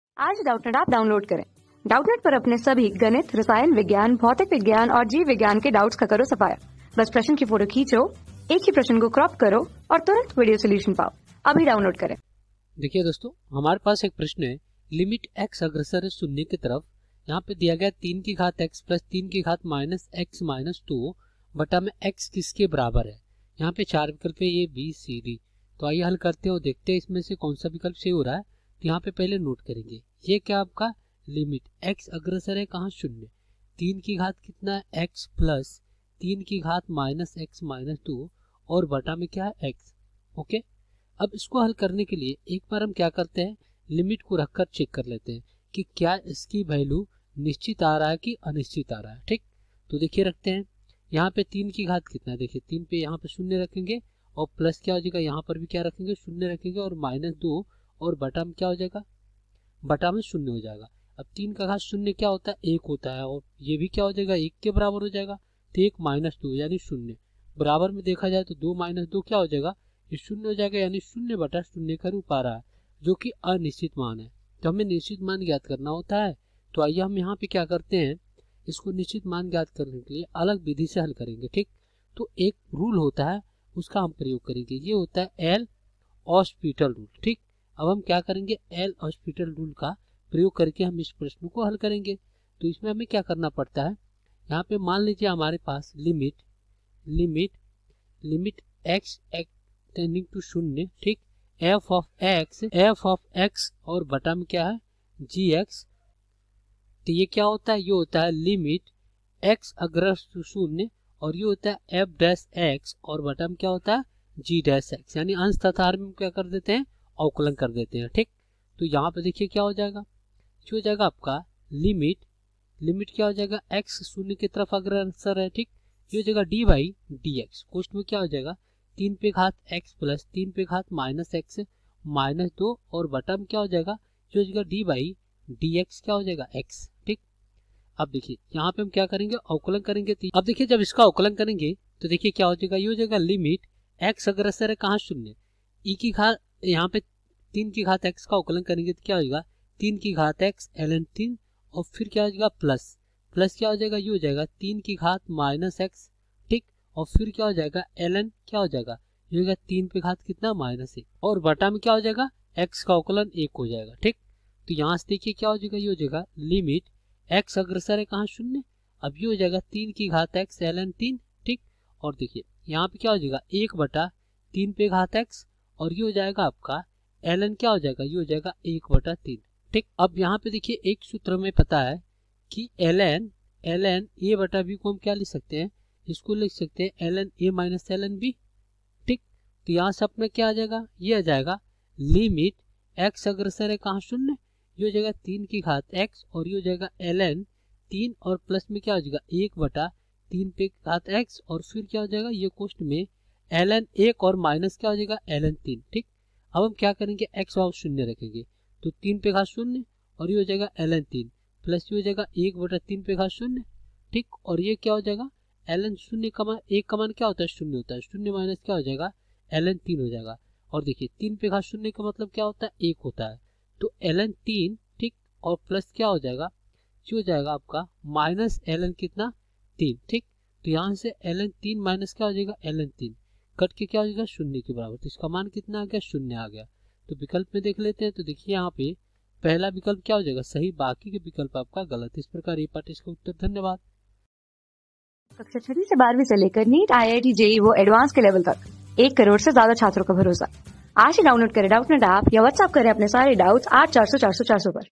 lim_(xto0)(3^(x)+3^(-x)-2)/x किसके बराबर है? | 12 | NDA 2020 | MATHS | NDA PREVIOUS YEAR HINDI
lim_(xto0)(3^(x)+3^(-x)-2)/x किसके बराबर है?
Class: 12
Subject: MATHS
Chapter: NDA 2020
Board:NDA

👉 Have Any Query? Ask Us.